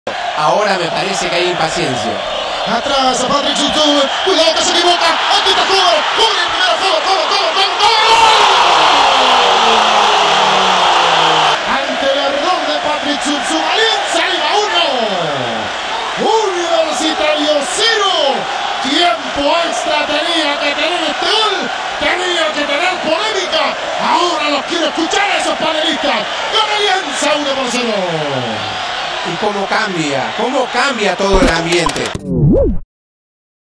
Alianza Lima vs. Universitario de Deportes: el golazo de Alejandro Hohberg por Torneo de Verano
¿Fue error o presión que induce al error de Patrick Zubzuck? Revisa este análisis táctico de Movistar Deportes.